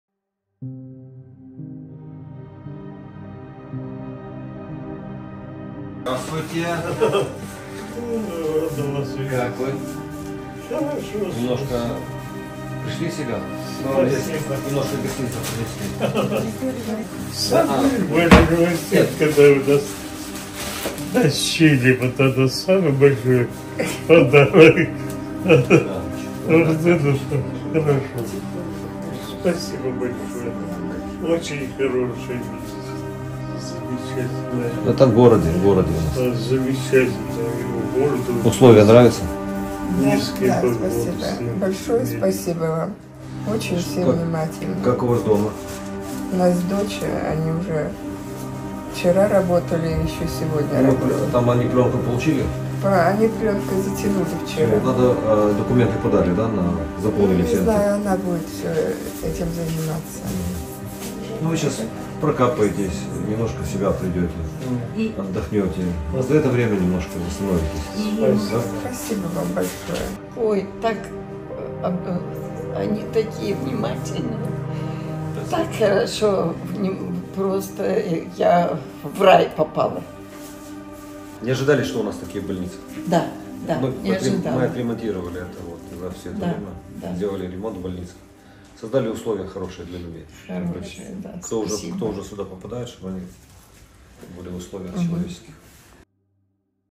Мер Одеси відвідав у лікарні постраждалих під час атаки 17 січня
Сьогодні відвідав у лікарні постраждалих внаслідок російської атаки 17 січня.

Було дуже приємно побачити у гарному стані сімʼю Шишкалових та їхню сусідку Емму Григорівну. Бажаю всім найскорішого одужання!

Усього в ту ніч постраждало 5 людей, з них четверо наразі в лікарнях. Їм надається вся необхідна медична допомога, і їхньому життю нічого не загрожує.